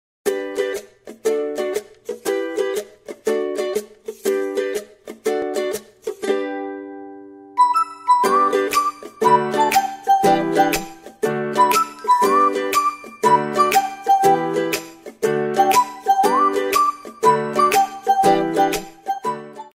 Recipe: Bee Pollen Milk
Ingredient:
Warm Milk     1 Cup
Bee Pollen      1Tbsp

材料：
温牛奶     1杯
蜂花粉     1汤匙

按赞&分享&评论来支持我们~